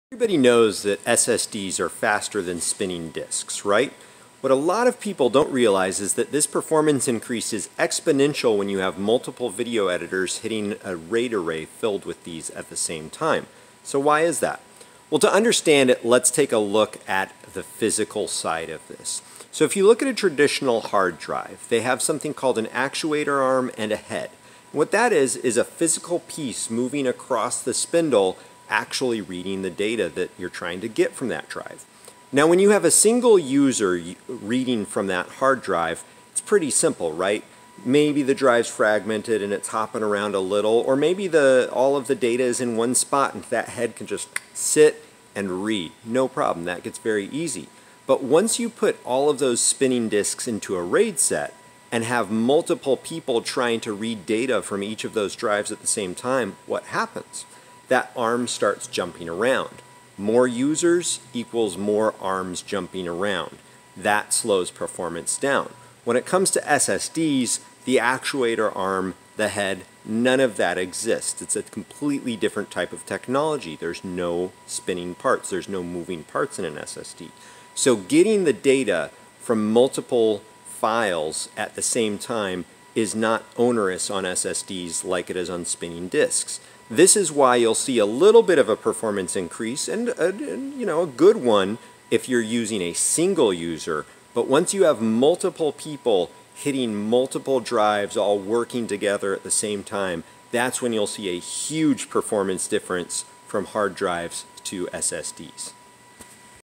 Why HHDs are slower for Video Teams | ProMAX ProTip 0017
Everybody knows that SSDs are faster than spinning disk right? What a lot of people don't realize is that this performance increase is exponential when you have multiple editors hitting a RAID array filled with them at the same time.

But once you have multiple people, hitting multiple drives, all working together at the same time that's when you'll see a huge performance increase from Hard Drives to SSDs.